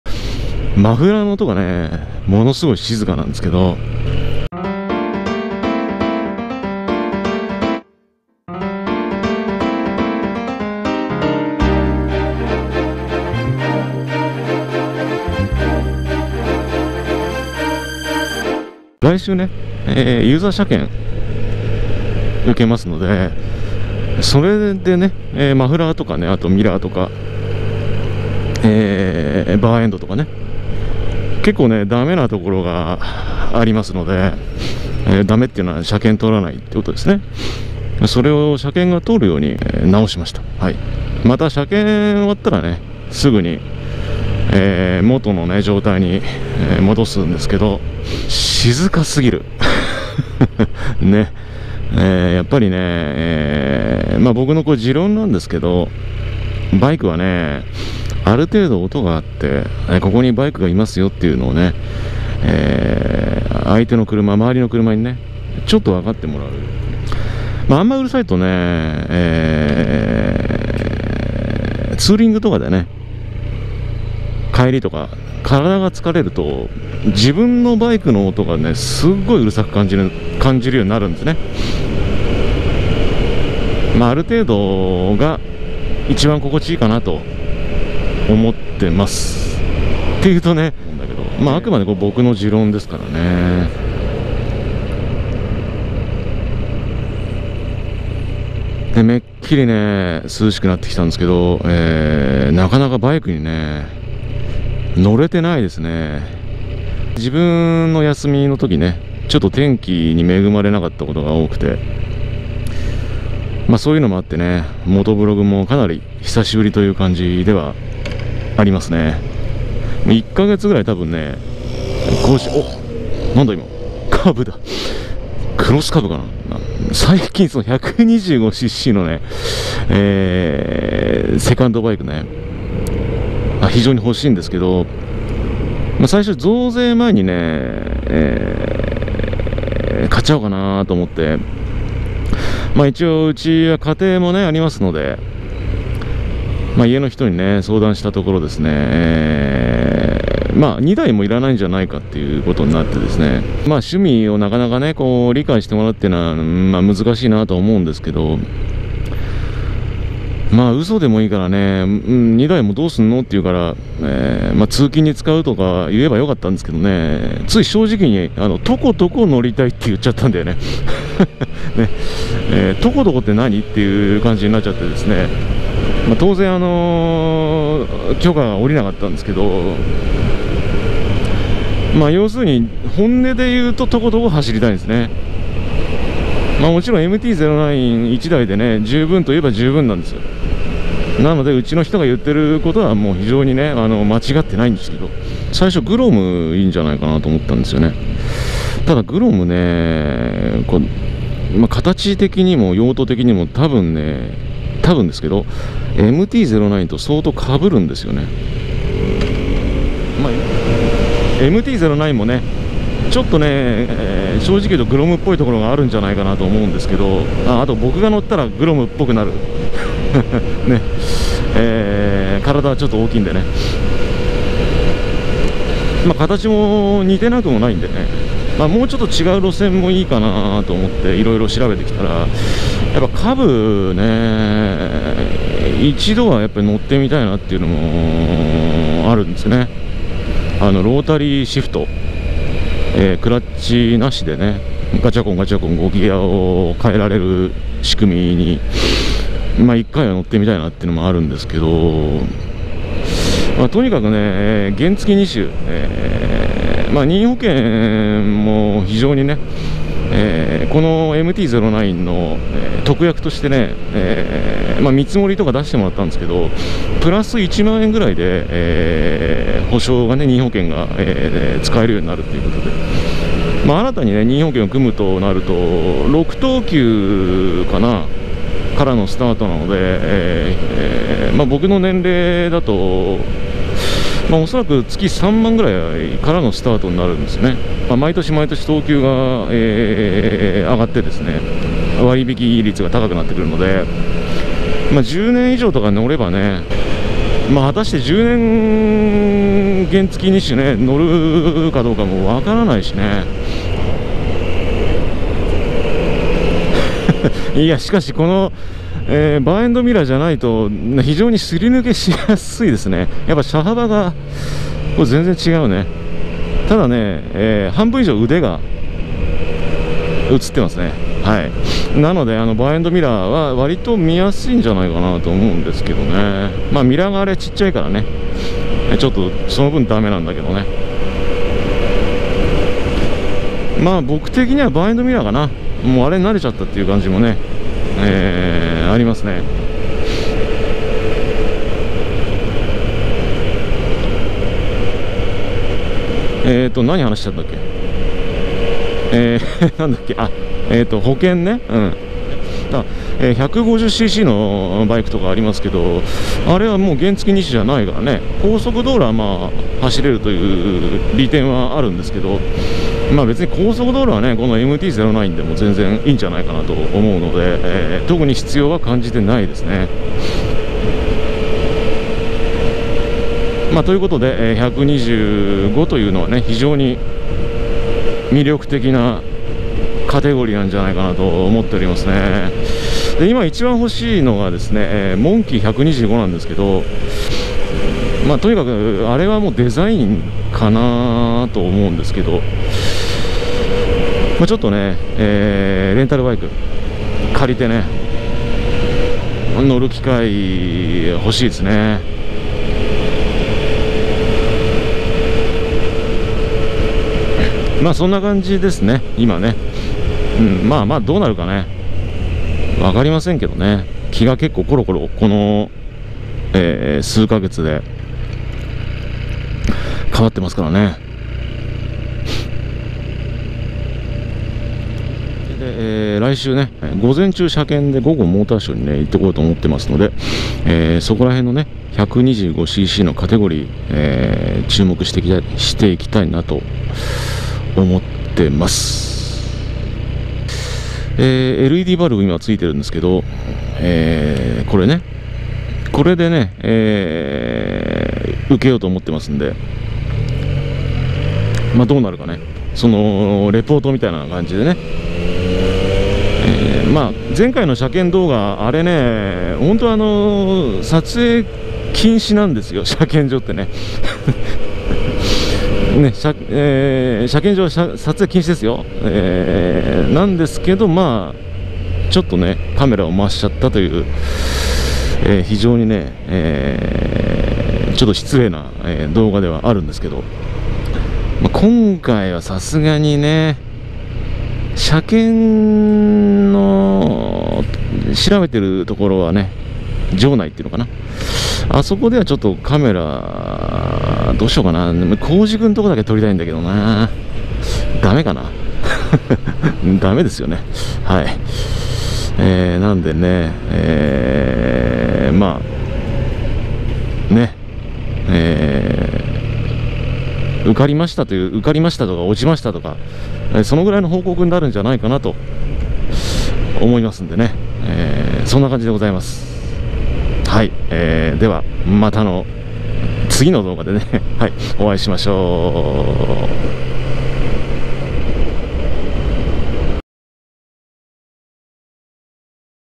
[雑談] 増車したいな [モトブログ]
来春には増車したいです、あくまで予定です。